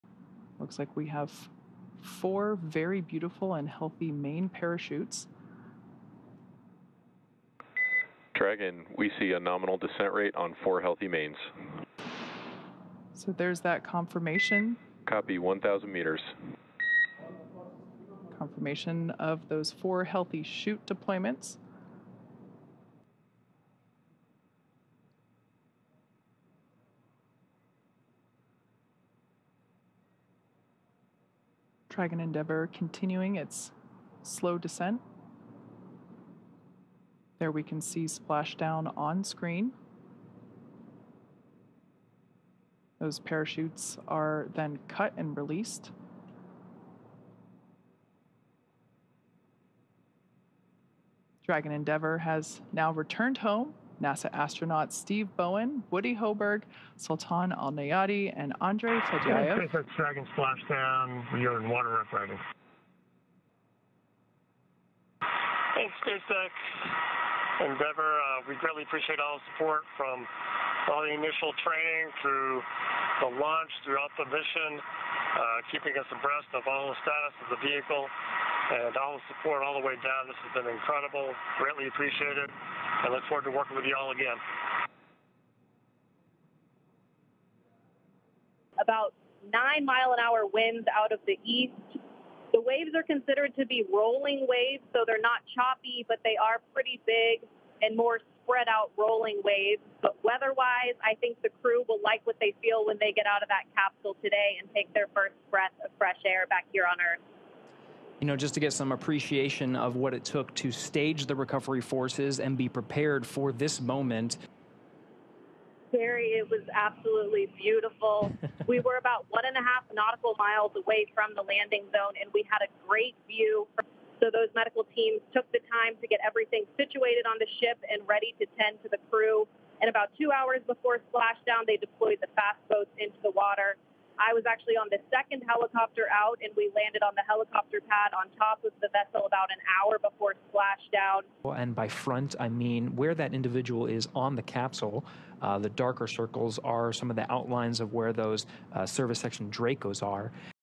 WATCH SpaceX capsule splash down into ocean as US, UAE and Russian astronauts return to Earth
Four astronauts returned to Earth early Monday after a six-month stay at the International Space Station.

Their SpaceX capsule parachuted into the Atlantic off the Florida coast.
Returning were NASA astronauts Stephen Bowen and Warren “Woody” Hoburg, Russia's Andrei Fedyaev and the United Arab Emirates' Sultan al-Neyadi, the first person from the Arab world to spend an extended time in orbit.

Before departing the space station, they said they were craving hot showers, steaming cups of coffee and the ocean air since arriving in March. Their homecoming was delayed a day because of poor weather at the splashdown locations.

SpaceX launched their replacements over a week ago.